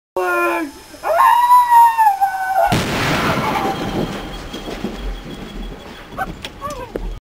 Luke Destroys Andrew
Luke accidentally blows up his brother with a match filled tennis ball.